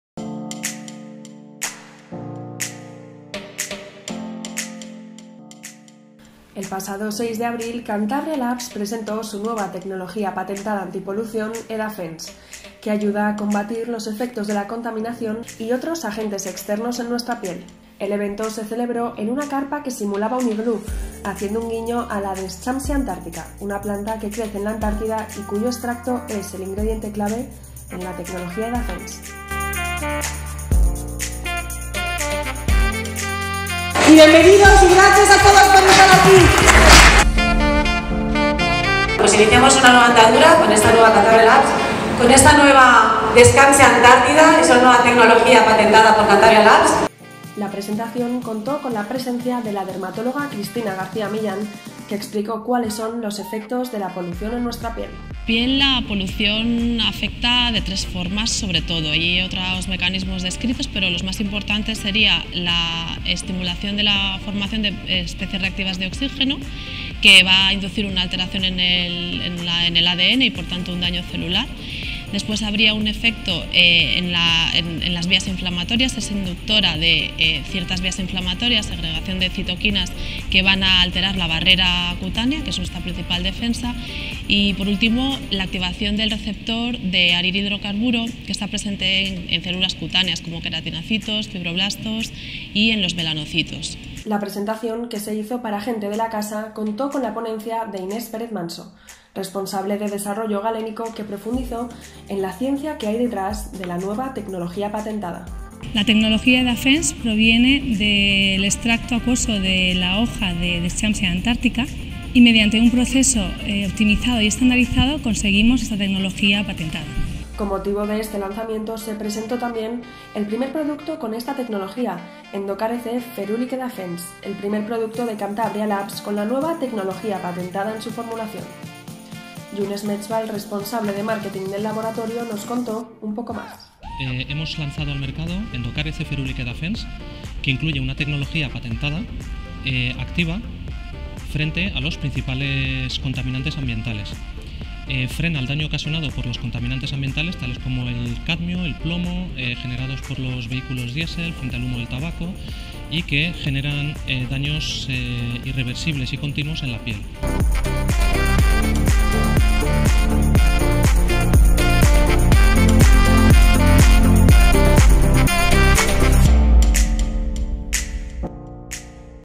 Cantabria Labs presenta su nueva tecnología EDAFENCE®
Cantabria Labs presenta su nueva tecnología patentada antipolución EDAFENCE®, que ayuda a combatir los efectos de la contaminación, la oxidación y otros agentes externos en nuestra piel.